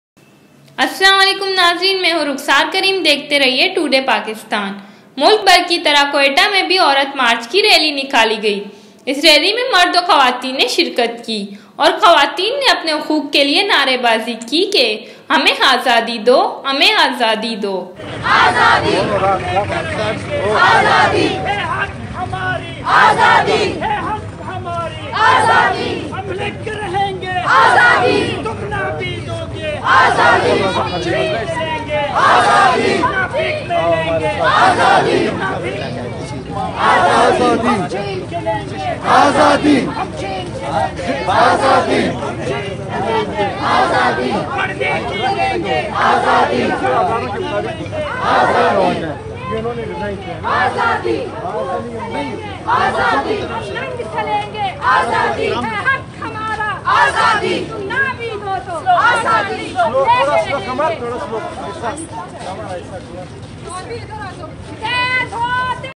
کوہٹہ میں عورت مارچ ریلی
خواتین کے عالمی دن کے موقع پر ملک بھر کی طرح کوہٹہ میں بھی عورت مارچ کی ریلی نکالی گئی۔جس میں مرد و خواتین نے شرکت کی